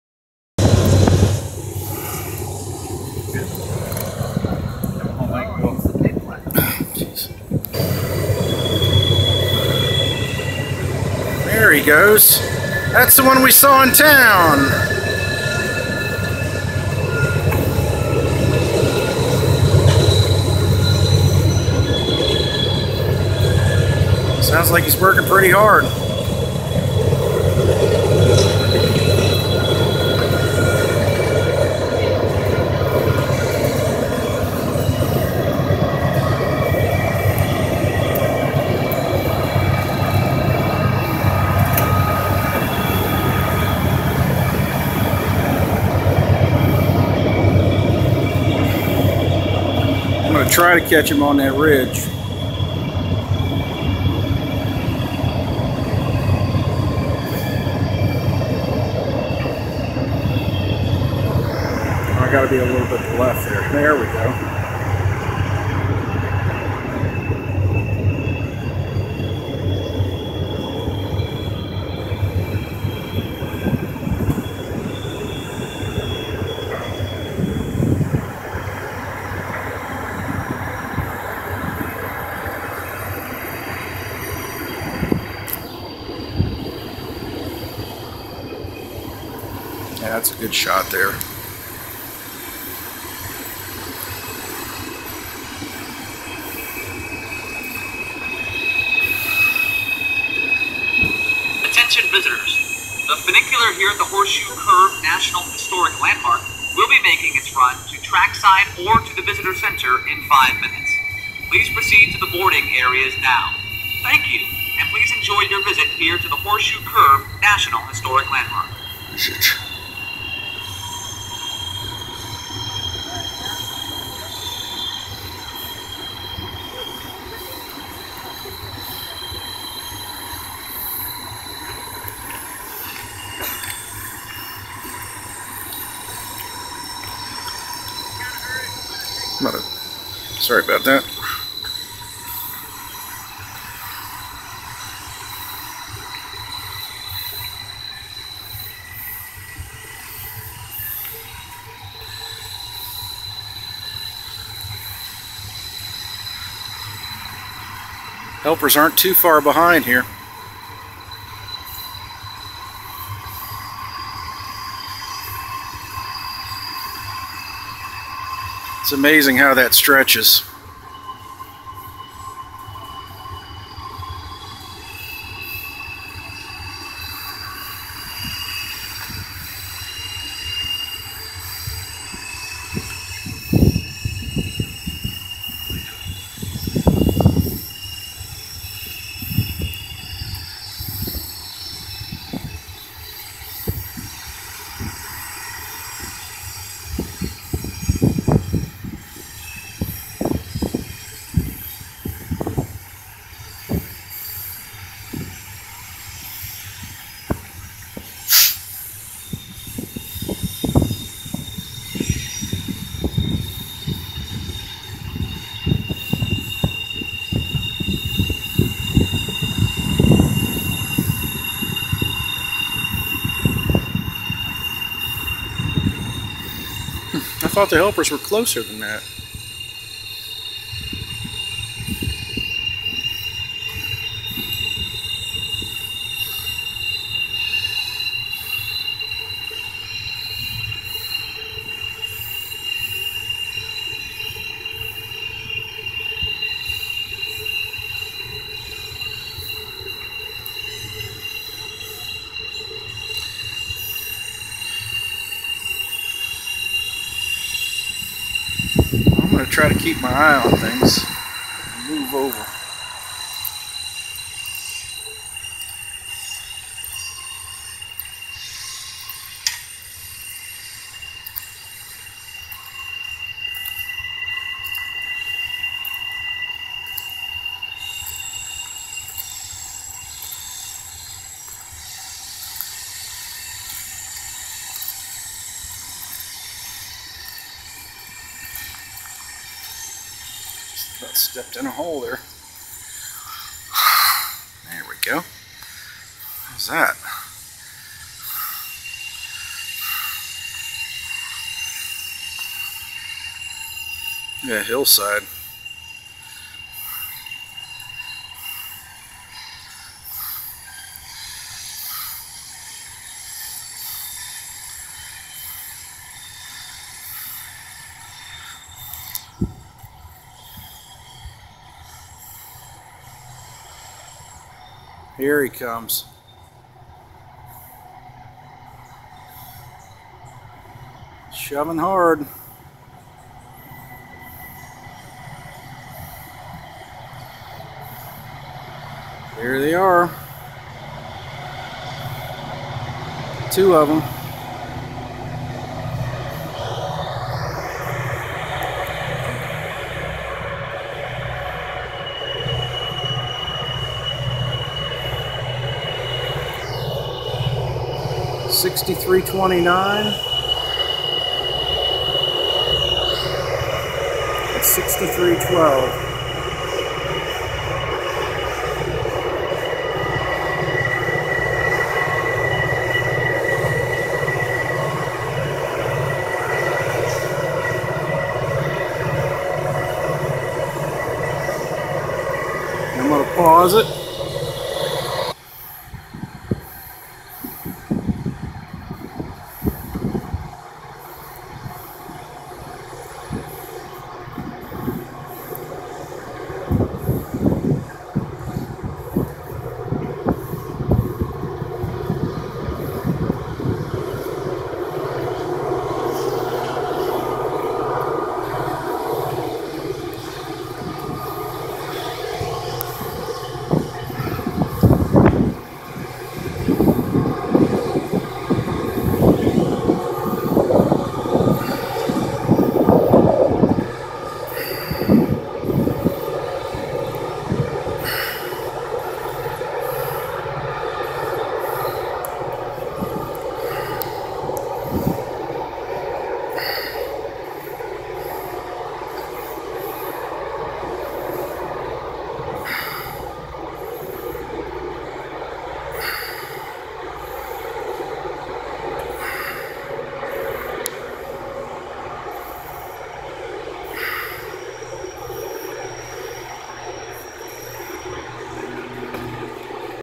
As we arrived in the parking lot, we saw a westbound Intermodal circling the Curve
Horseshoe Curve, PA Friday, August 12th, 2016 about 11:30 a.m.. If you have not been to Horseshoe Curve recently within the last three or four months , you will be in for a pleasant surprise because a contractor came through and trimmed all of the tree line circling the hill.